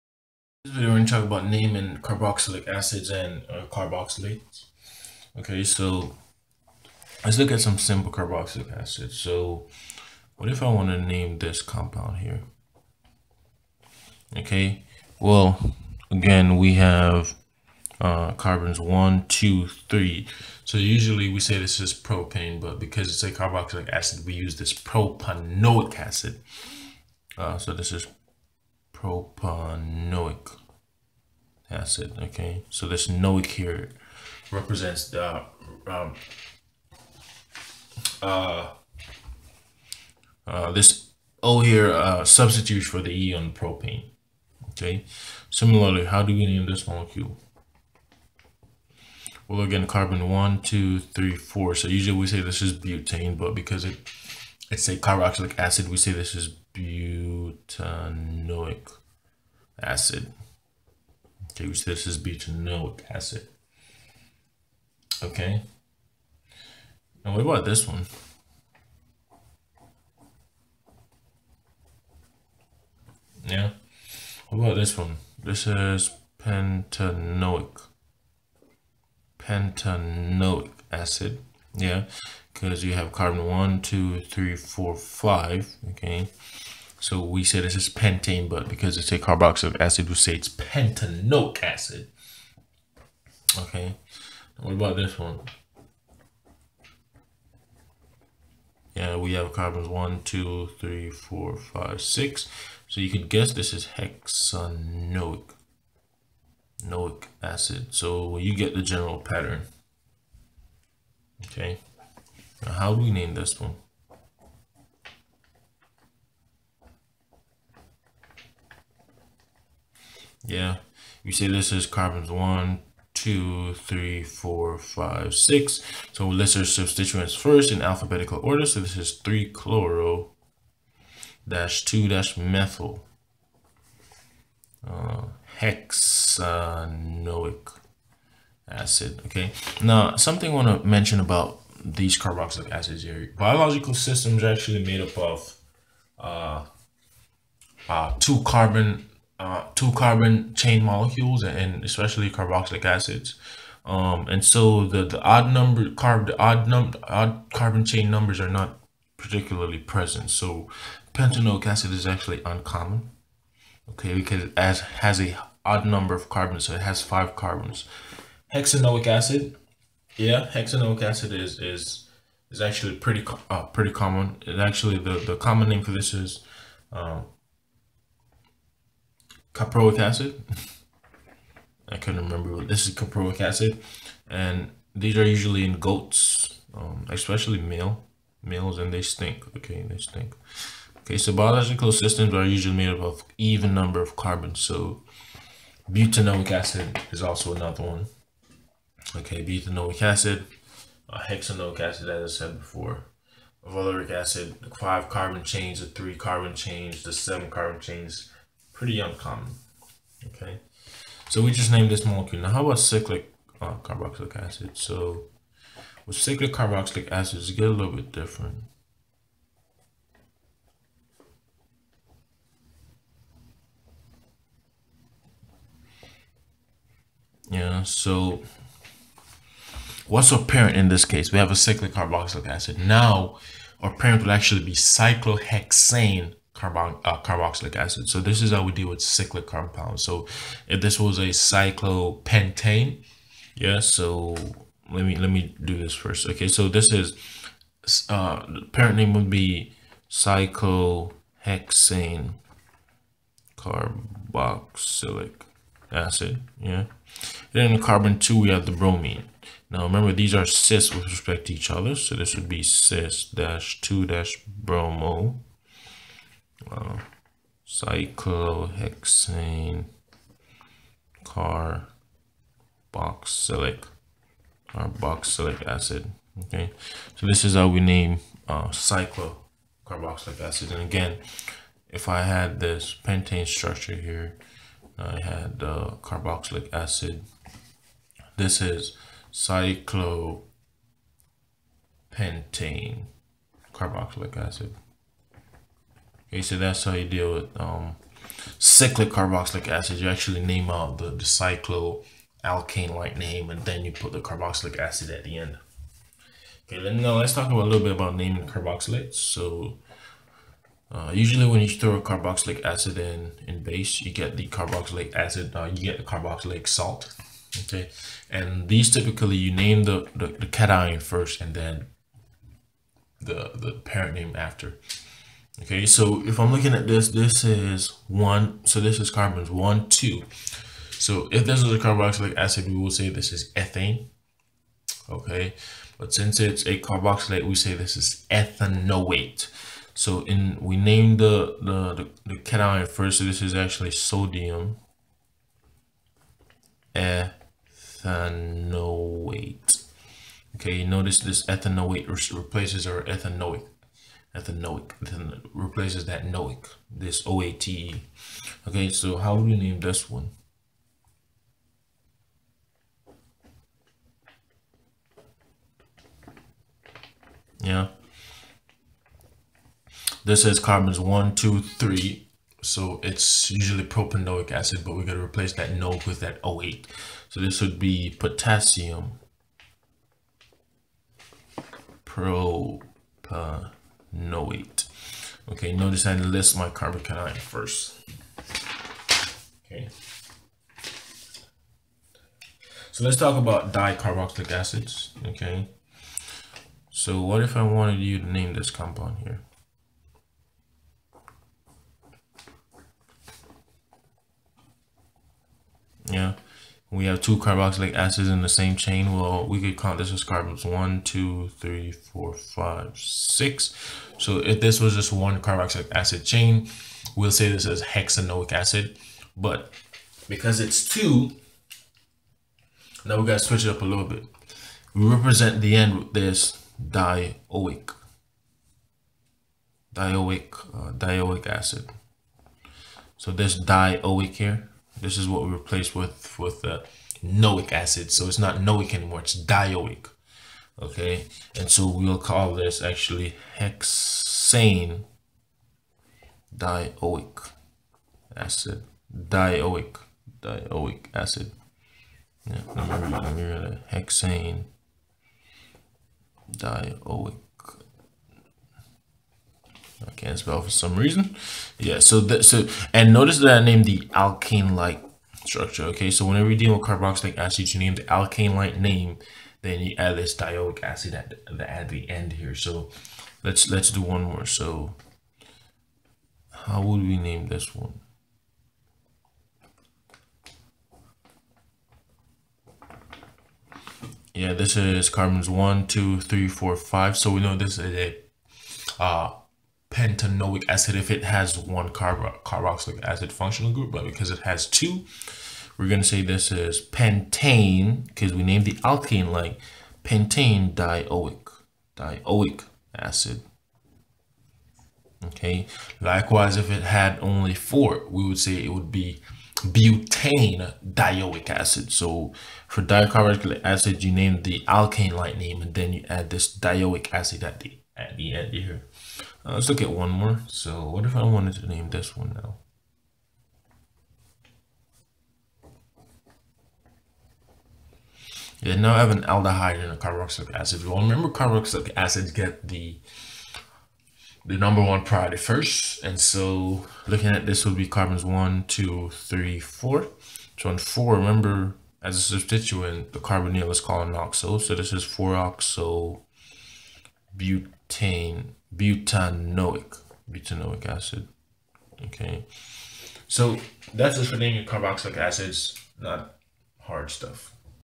Naming Carboxylic Acids and Carboxylates (Nomenclature)
This video puts emphasis on the nomenclature of Carboxylic acids as well as Carboxylates.